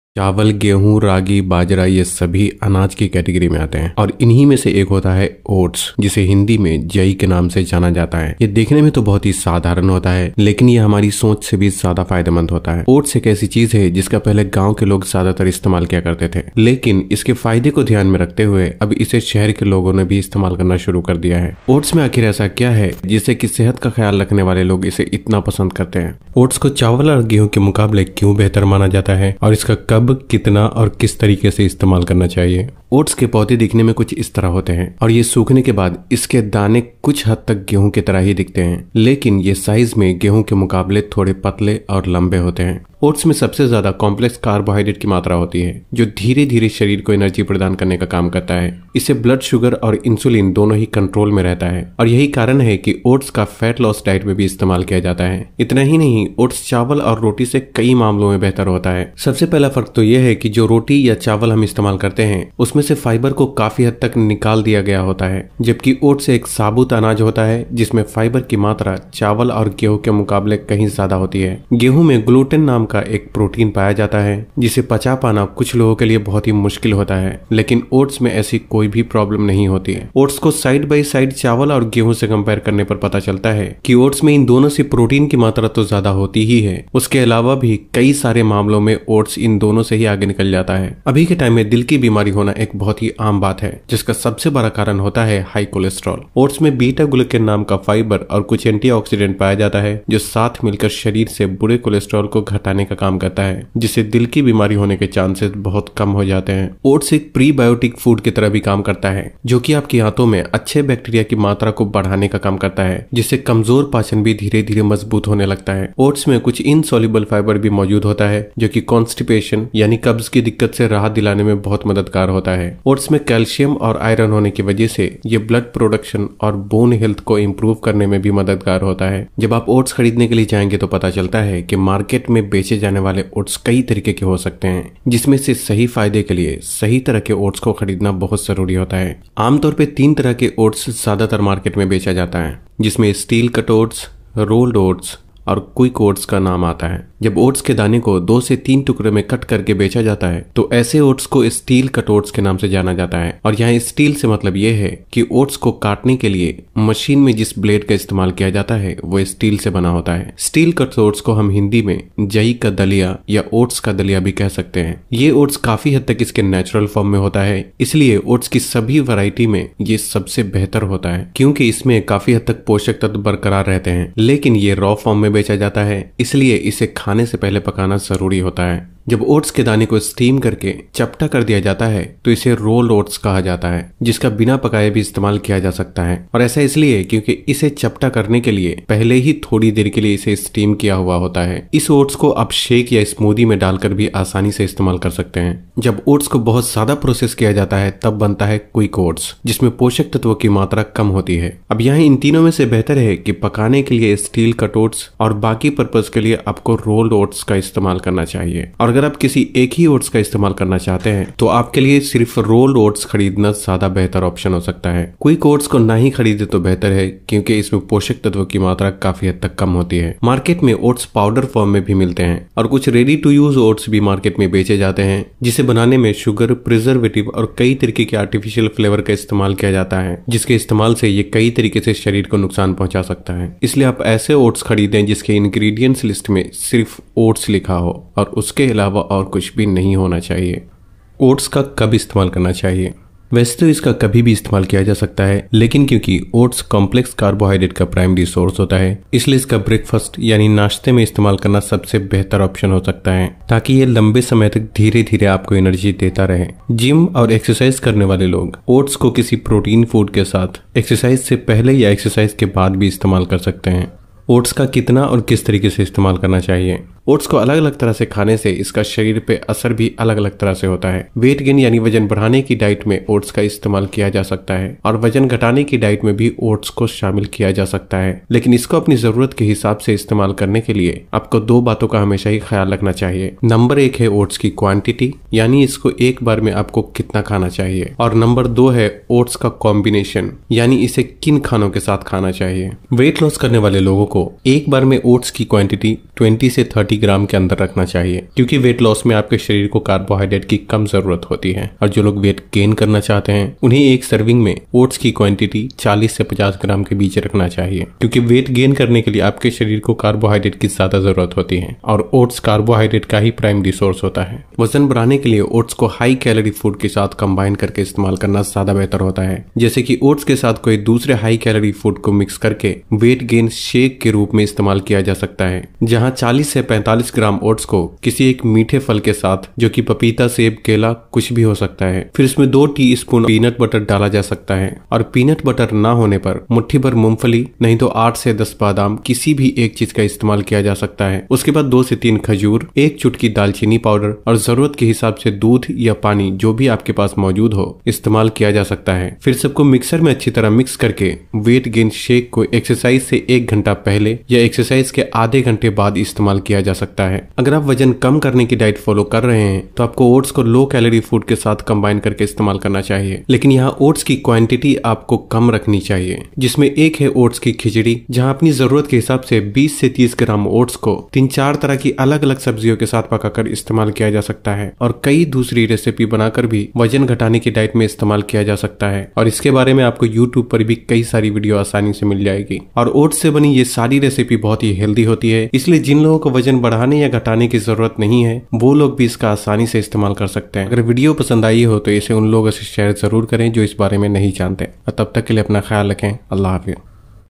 Oats 🔥 एक Super Food - खाने का सही तरीका
Duniyan bhar me kai trah ke anaj paye jate hain jaise ki chawa, gehu, ragi or bajra inhi me se ek hota hai oats jise hindi me jai ke nam se bhi jana jata hai...

Oats ek aisi chij hai jiska pehle gaon ke log jyada istemal krte the lekin ab ise shaher me rhne wale logon ne bhi istemal krna shuru kr diya hai...halaki ab bhi bahot ye bat bhi thik se nhi jante ki oats kya hota hai lekin jo log sehat or fitness ke bare me jankari rakhte hain unhe oats ke bare me jarur pta hota hai...

oats me akhir aisa kya hai jisse ki sehat ka khayal rakhne wale log ise itna pasand krte hain...

Iska kab kaise or kitna istemal krna chahie

Oats ko chawal or genhun ke mukable kyun behtar mana jata hai...

Or sabse jaruri bat ki wajan badhane or ghatane wale log ise kis trah se apne khane me shamil kr sakte hain ...